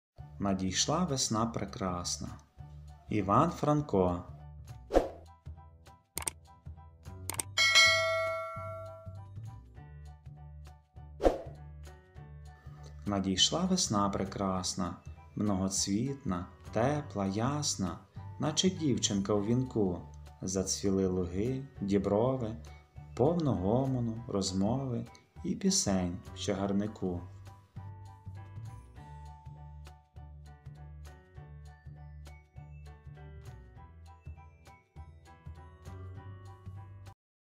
Надійшла весна прекрасна... – Іван Франко
Пропоную для вашої уваги це відео із твором українського автора Івана Франка  "Надійшла весна прекрасна..." Бажаю приємного перегляду та задоволення від прослуховування.

Надійшла весна прекрасна,
многоцвітна, тепла, ясна,
наче дівчинка в вінку.
Зацвіли луги, діброви,
повно гомону, розмови
і пісень в чагарнику.